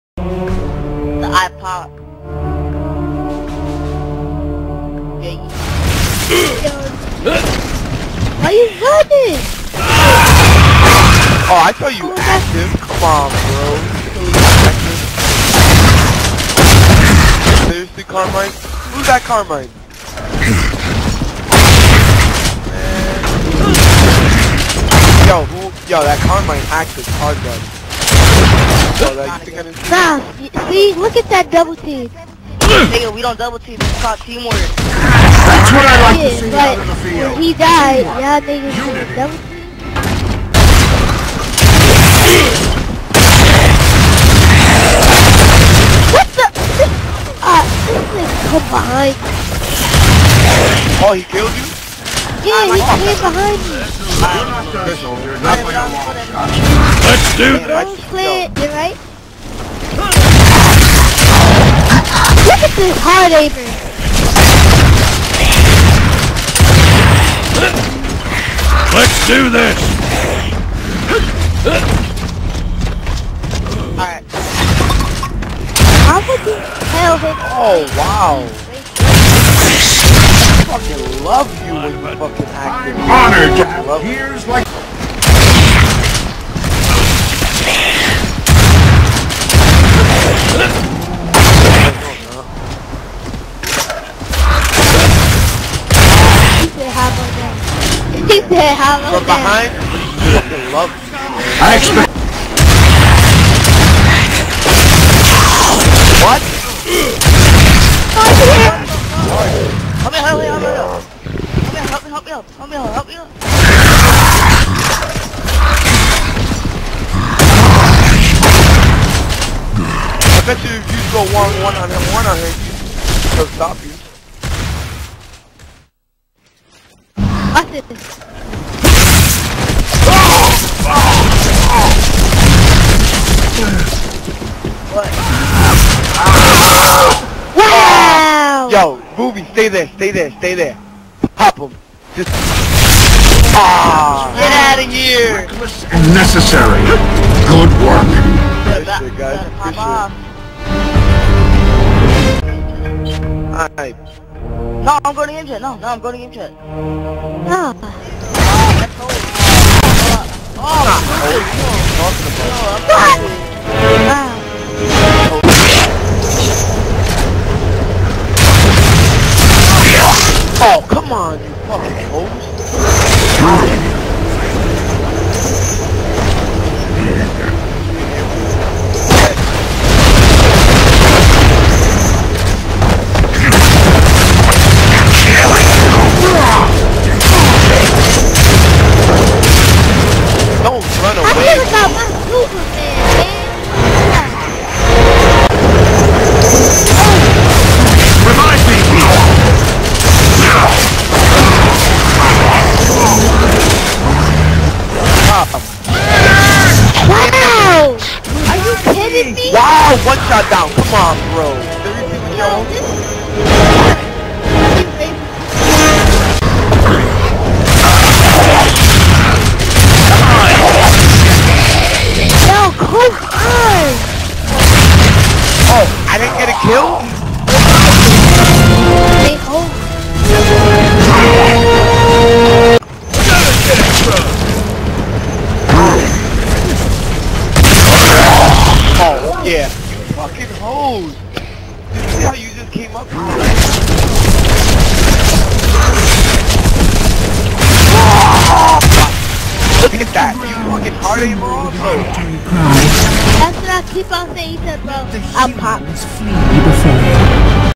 JEx42 VS xBRoZx(FUNNY)
2V2 LOL GG THEY WERE HILARIOUS THATS WHY I AINT PUT NO MUSIC CUZ U HAVE TO HEAR THEM BITCHING LMAO